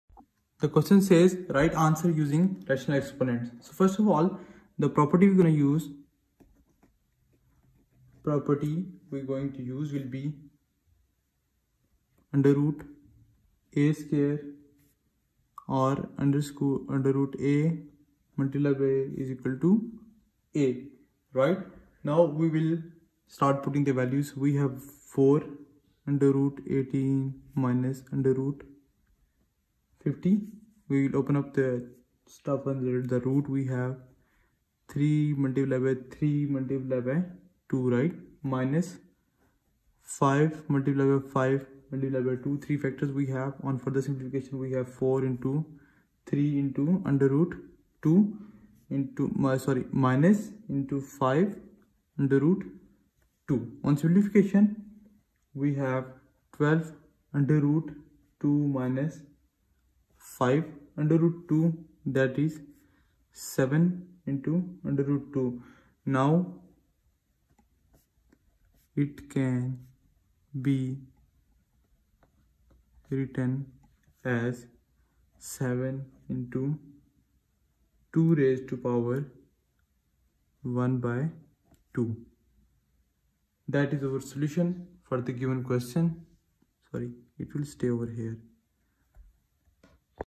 Simplify the expression:Write the answer using rational exponents,Ka ... | Plainmath
Solution to Algebra question: Simplify the expression:
Write the answer using rational exponents,Ka \sqrt[n]{m}, would look like a(m^{(^1/_n)}):
4 \sqrt{18}\ -\ \sqrt{50}